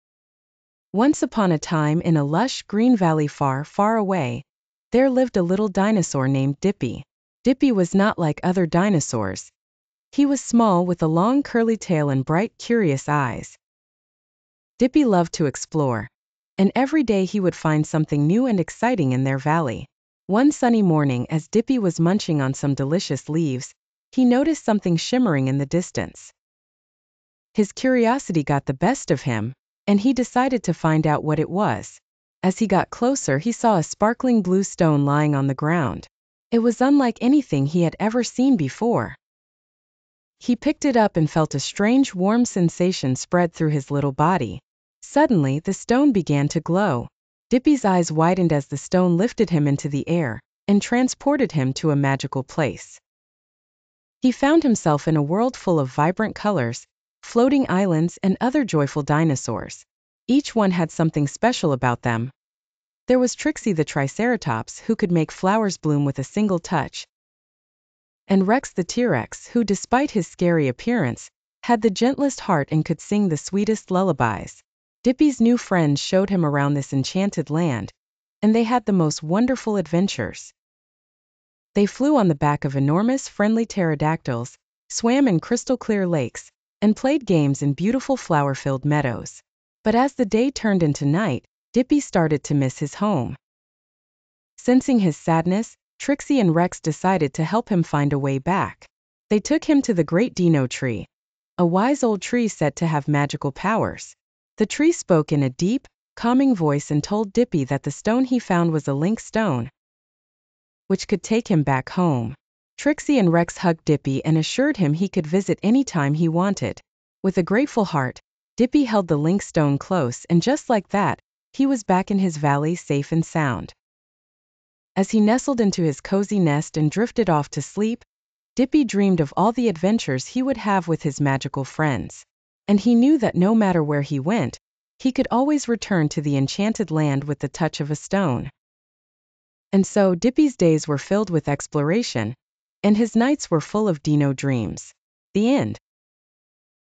Dippy's Magical Adventure
In a vibrant valley, a curious little dinosaur named Dippy discovers a shimmering blue stone that transports him to a magical world filled with friendly dinosaurs and enchanting adventures. Alongside Trixie the Triceratops and Rex the T-Rex, Dippy explores floating islands and crystal-clear lakes. When he longs for home, the wise Great Dino Tree reveals the stone's power to return him. Dippy learns he can revisit this magical land anytime, filling his days with exploration and his nights with dreams.